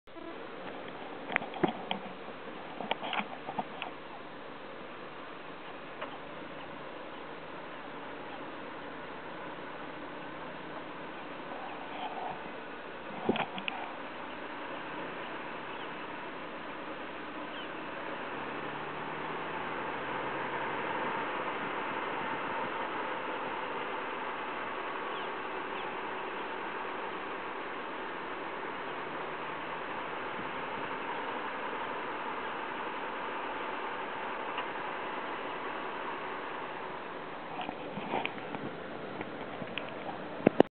moosecalf chasing her car mother
Newborn moosecalf without her mother filmed near Öster Malma in Södermanland, heartbreaking sweet little calf. In my first video, "Newborn moosecalf crying for her mother", the moosecalf is crying for her mother...